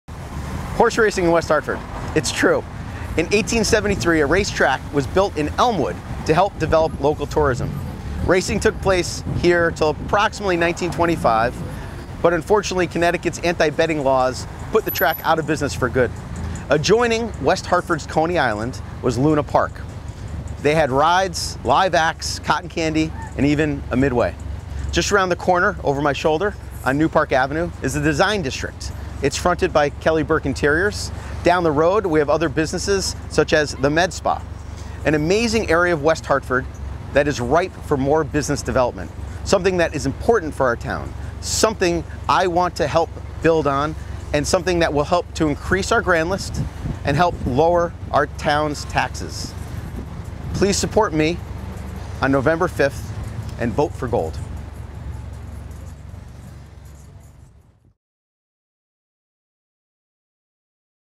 Horse Racing in West Hartford
Horse racing, in West Hartford?  Lee Gold, candidate for West Hartford Town Council, explains how, not only did West Hartford once have a horse track, but even had it's own version of Coney Island - watch to find out more!